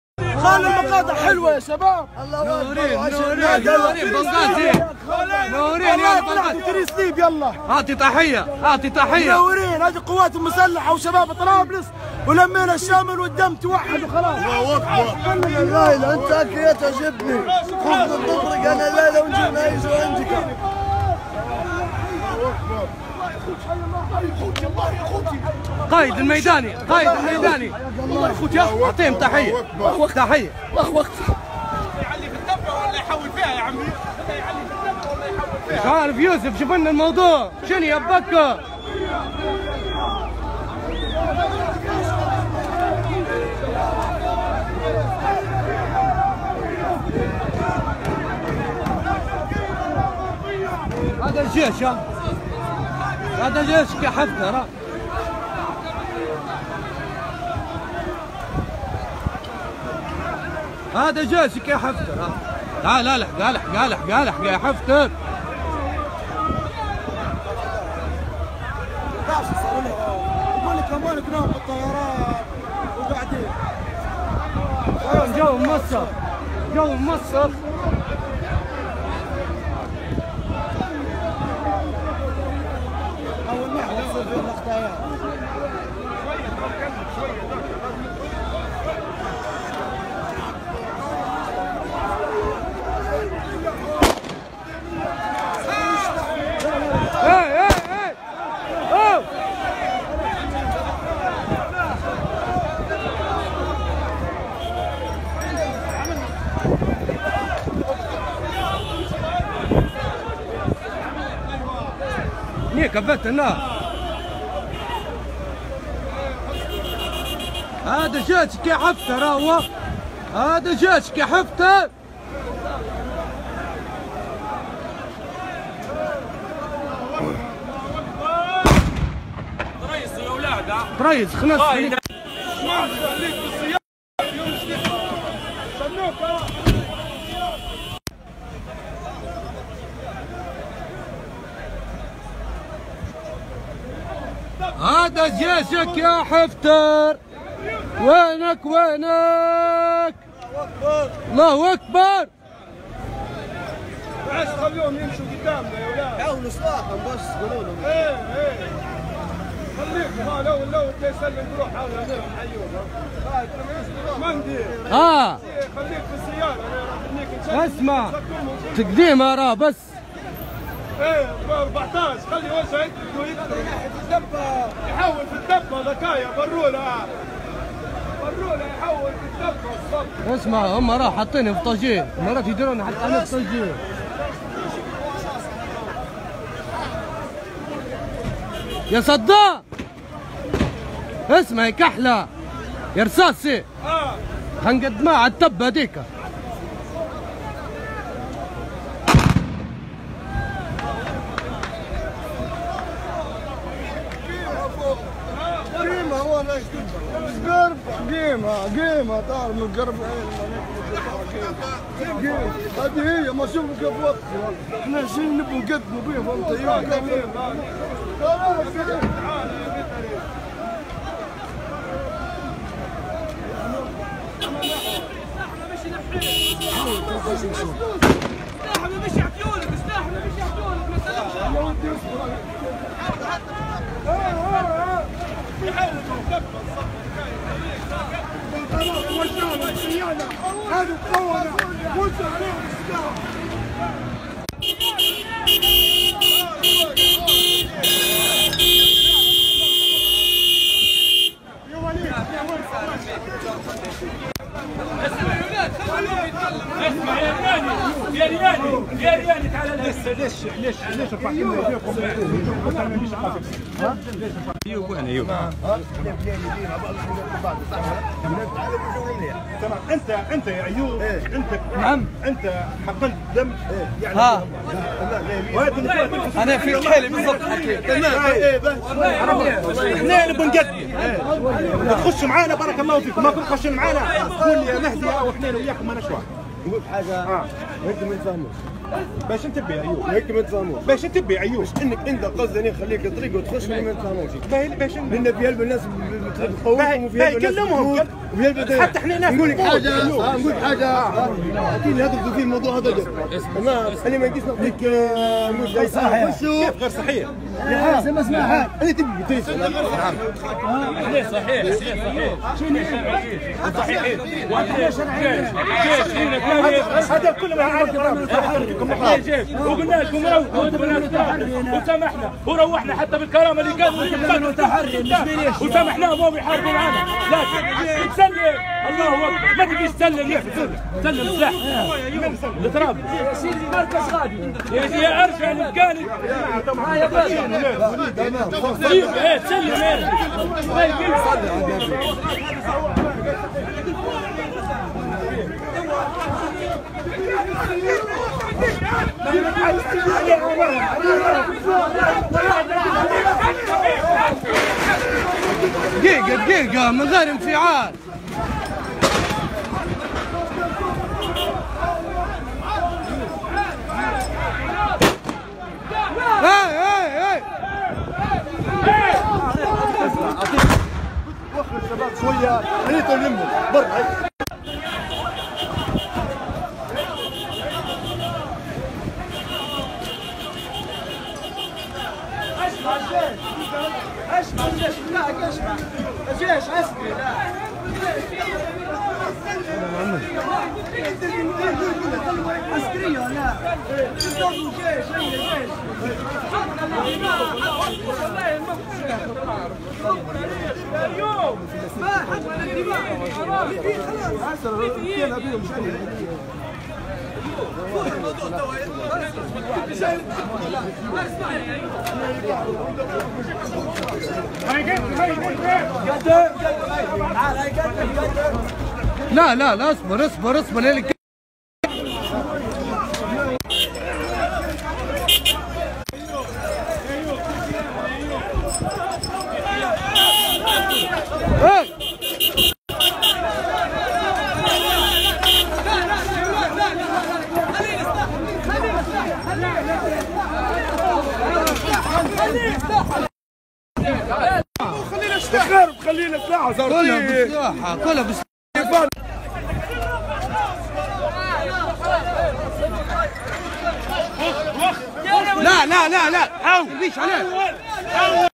ليبيا محور عين_زارة (الكحيلي) 11-04-19 : كتيبة محاصرة التابعة لـ حفتر سلمت نفسها للثوار
ليبيا 11-04-19 محور الكحيلي_عين_زارة : الكتيبة الثامنة التابعة للمدعو حفتر سلمت نفسها لقوات الثوار